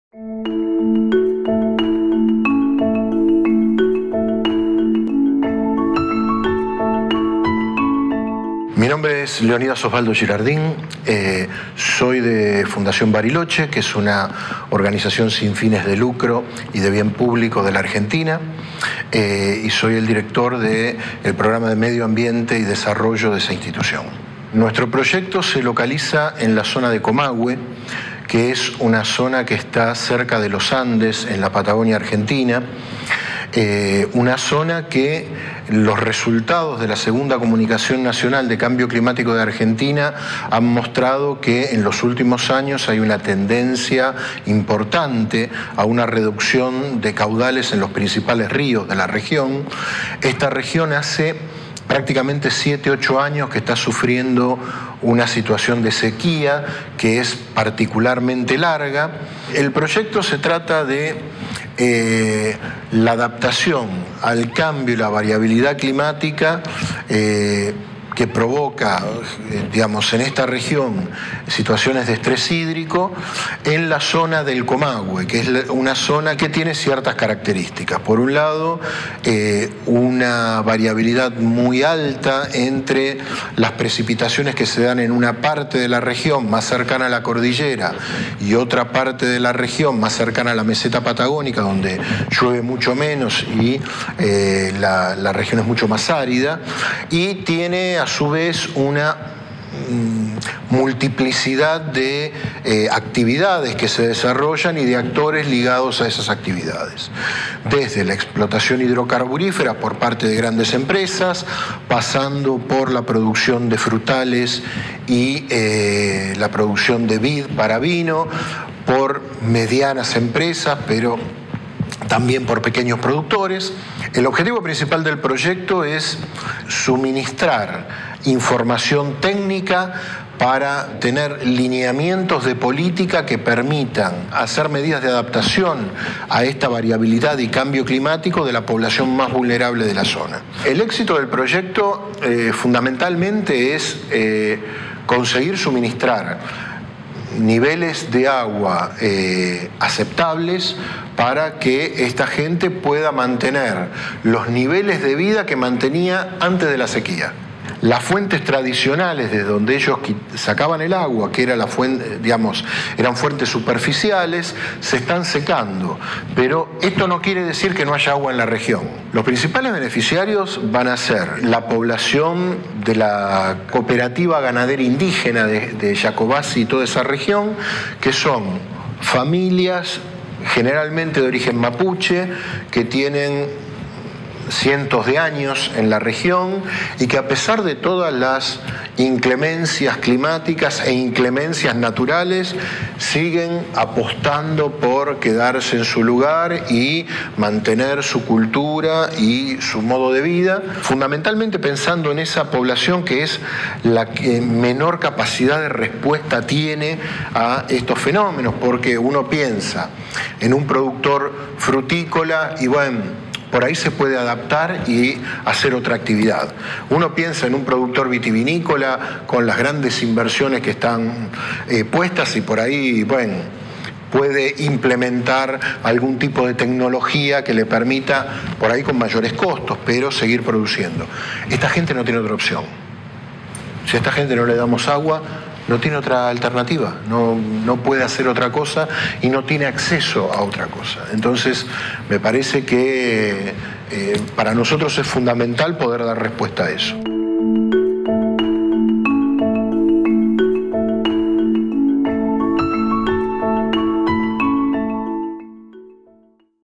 In Conversation with Osvaldo Girardin
How is increasing water stress affecting different users in northern Patagonia? Osvaldo Girardin of Fundación Bariloche explains the impacts that climate change is having for different sectors and for native communities in Argentina's Comahue Region.

The In Conversation video series brings to light the range of development research carried out by IDRC's partners, through short interviews with project teams.